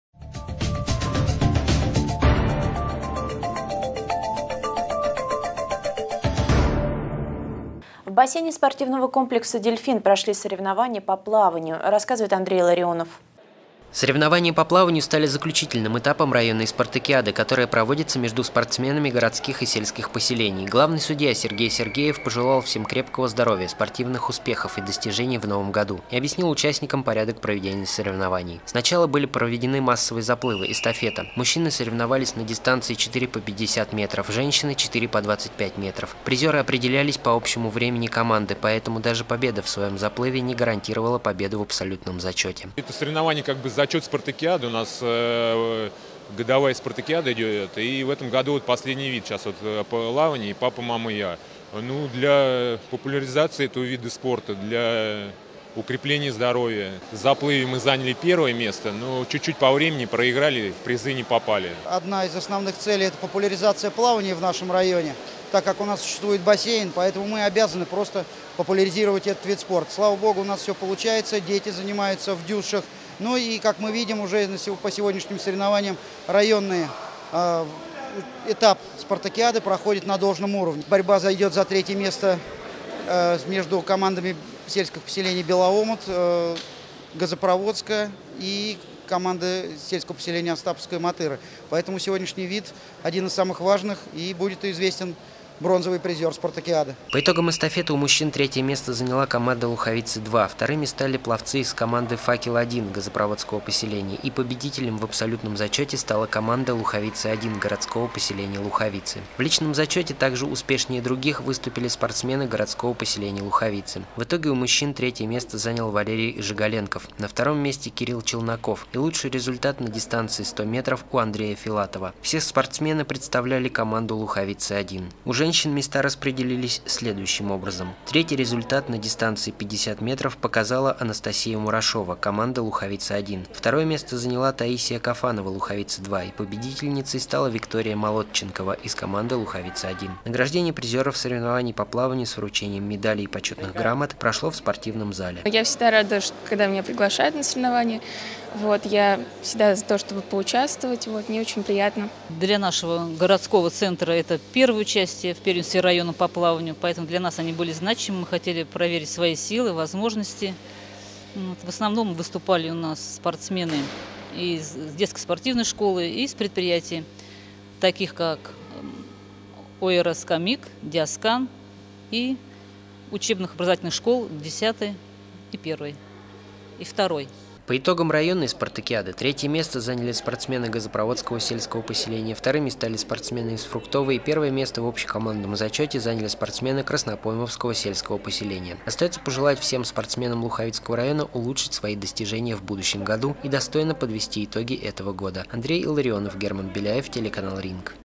В Луховицах прошли соревнования по плаванию
Соревнование по плаванию завершило районную спартакиаду, которая проводится между спортсменами городских и сельских поселений Луховицкого района.